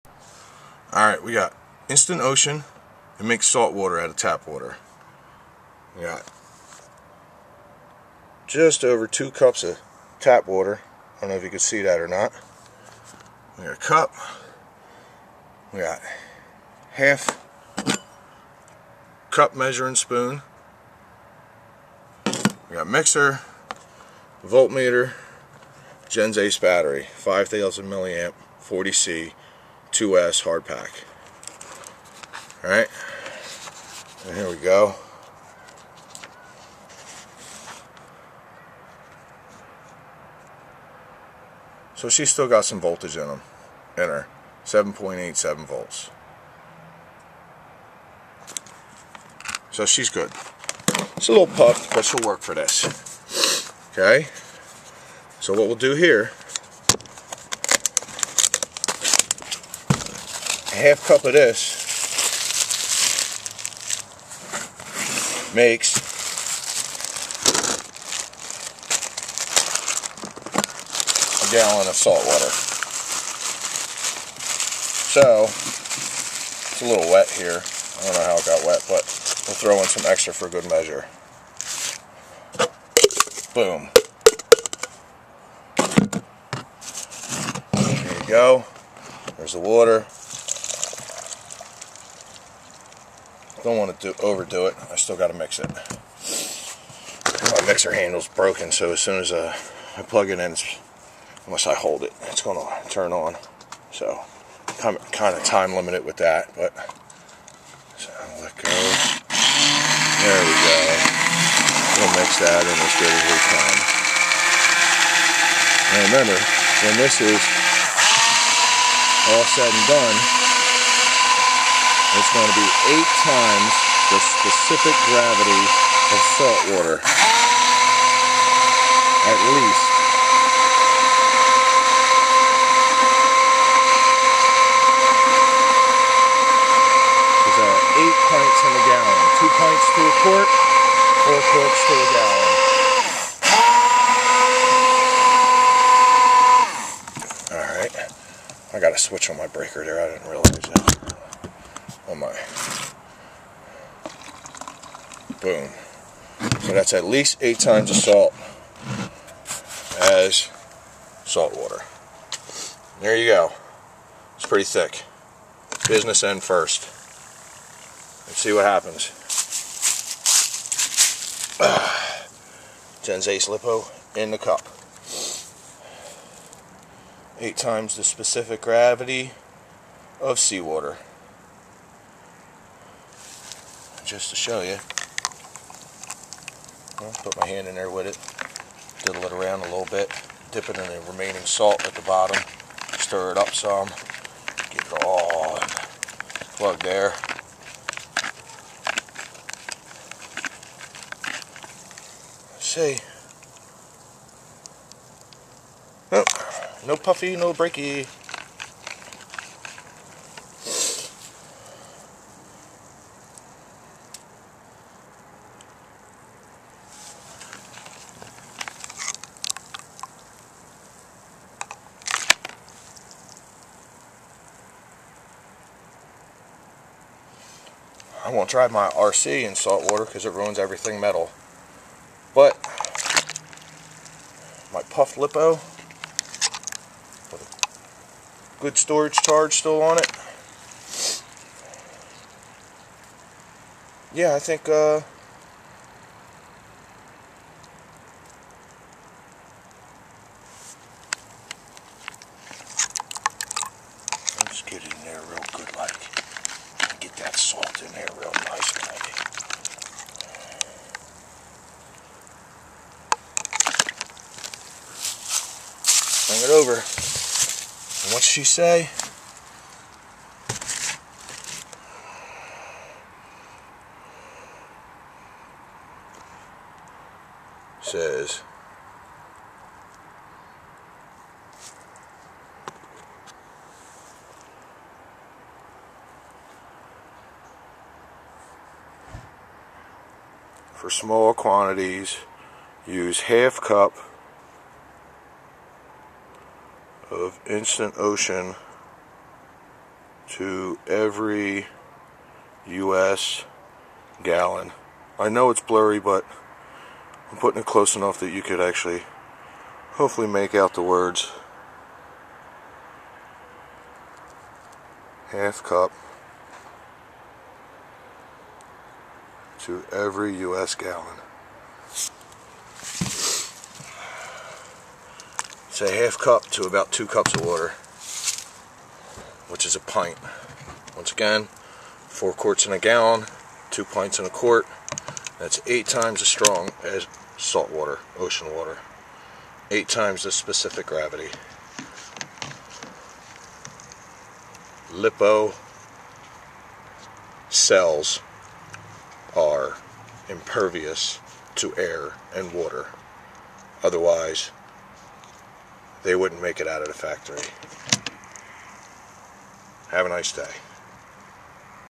lipo salt water dunk
as you can see lipo packs will be fine in salt water. the cells are protected by a plastic seal that's impervious to air and water.